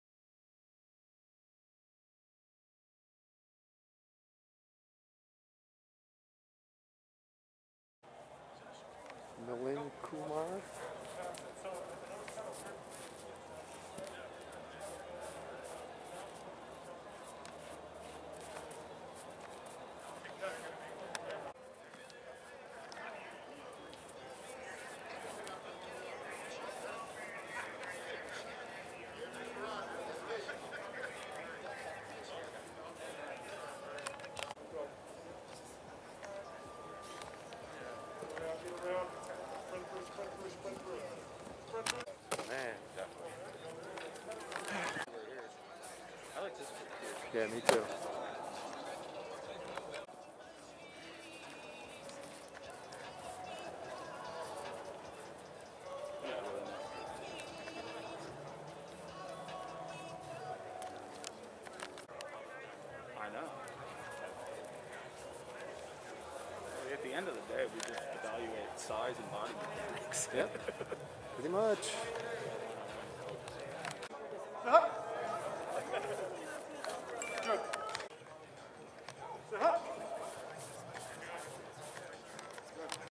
EDGYTIM The Opening: 2022 Schaumburg OL Milind Kumar
EDGYTIM brigs you these exclusive linemen workout videos from The Opening 2020 regional in Crown Point Indiana.